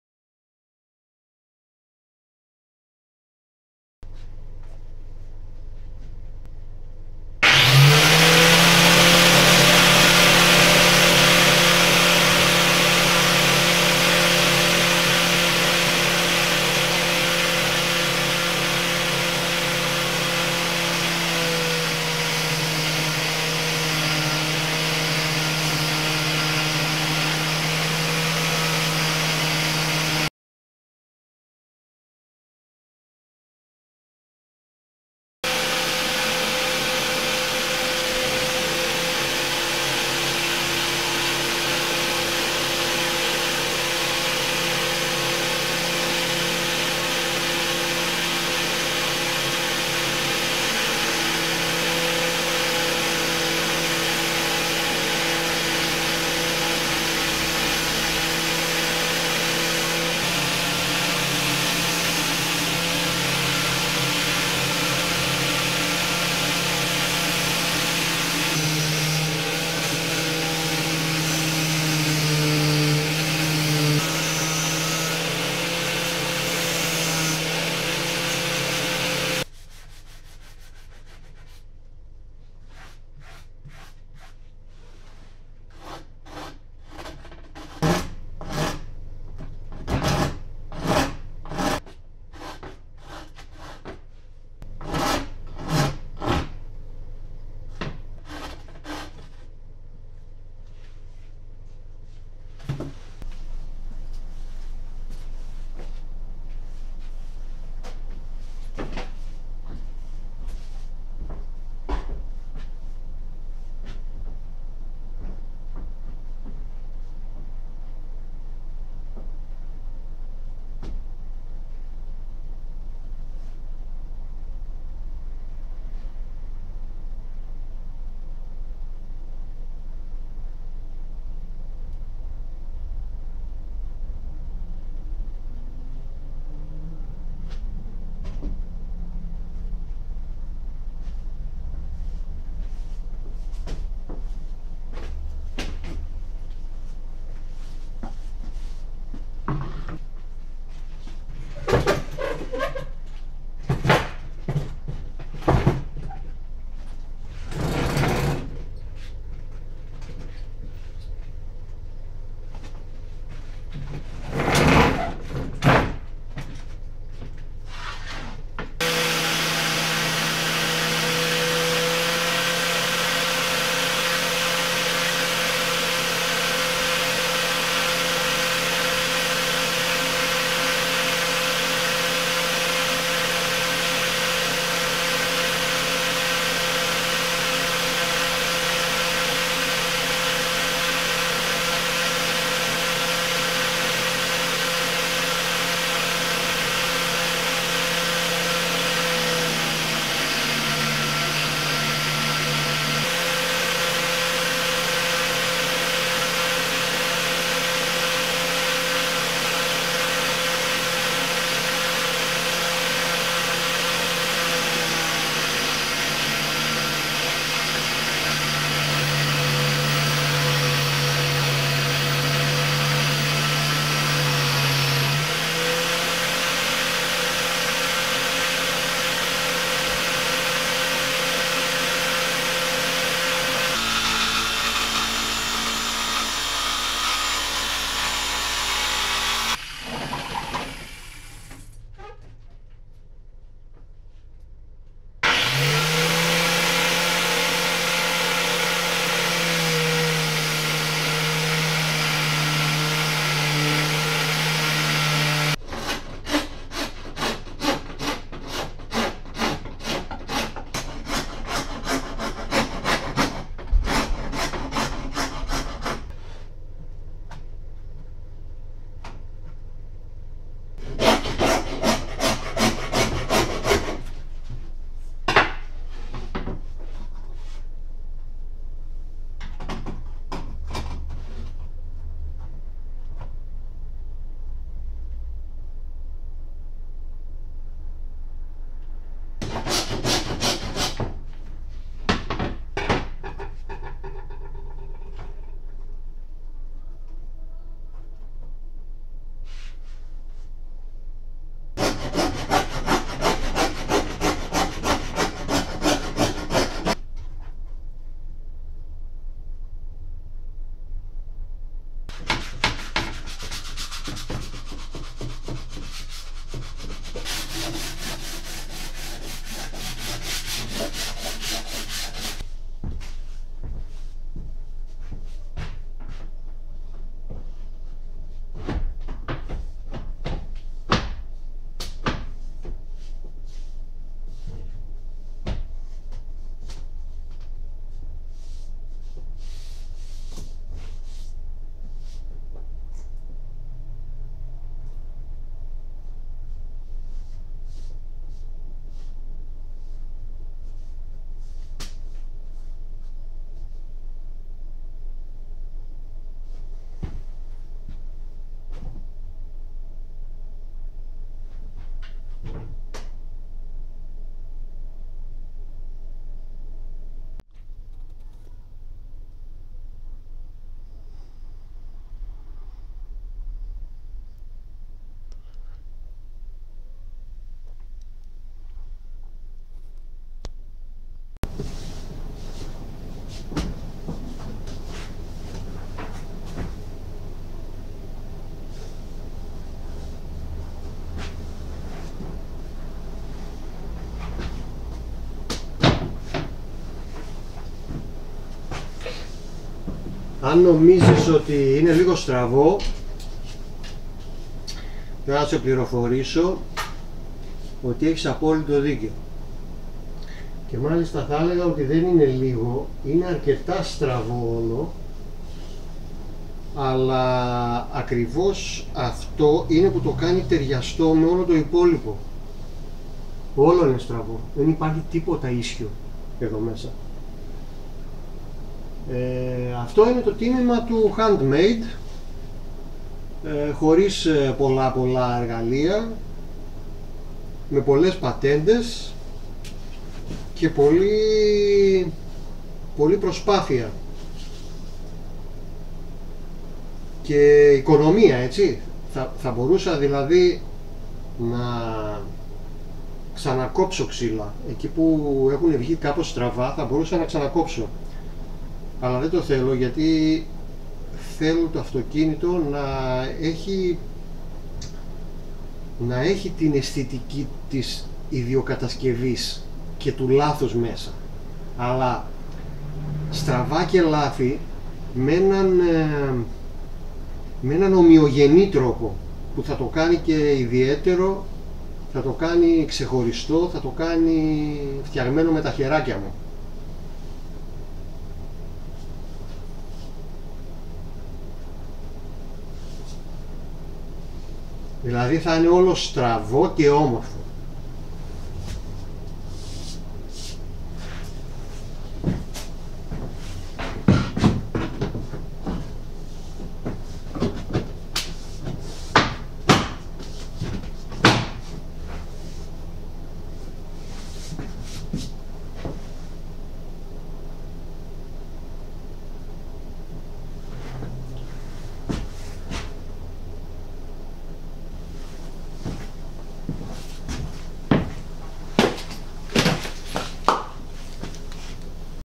Ντουλάπι για τον χώρο πάνω από την καμπίνα οδήγησης
Σε αυτό το βίντεο δείχνω τον τρόπο που χρησιμοποιώ τα απλά εργαλεία μου για να διαμορφώσω την πρόσοψη του ντουλαπιού πάνω από την καμπίνα οδήγησης. Είναι μια περιοχή με αρκετά μεγάλες διαστάσεις που μπορεί να πάρει πολλά από τα ρούχα μας.